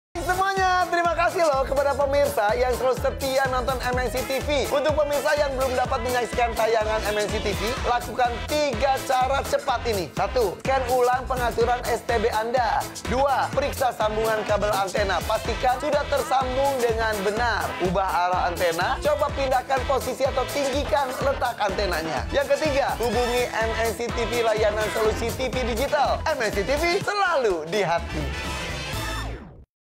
Hai Hai Keluarga Indonesia, Jangan Lupa Untuk Atur STB, Ya! ✨
Hai hai Keluarga Indonesia !! ✨⁣
Untuk kalian yang belum bisa menyaksikan tayangan MNCTV dapat lakukan 3 cara cepat ini :⁣
1. 𝐒𝐜𝐚𝐧 𝐮𝐥𝐚𝐧𝐠 pengaturan STB (Set Top Box) Anda.⁣
2. Jika tayangan tetap tidak terlihat, ikuti Langkah berikut :⁣
- Periksa sambungan kabel antena, pastikan sudah tersambung dengan benar.⁣
- Ubah arah antena, coba pindahkan posisi atau tinggikan letak.⁣
3. Hubungi MNCTV Layanan Solusi TV Digital melalui whatsapp ke 𝟎𝟖𝟓𝟔𝟗𝟎𝟎𝟑𝟗𝟎𝟎 jika masih belum berhasil.⁣
Saksikan terus program-program kesayanganmu hanya di MNCTV Selalu Di Hati. ❤️ Dan jangan lupa saksikan terus Family100 SETIAP HARI pukul 19.30 WIB hanya di MNCTV.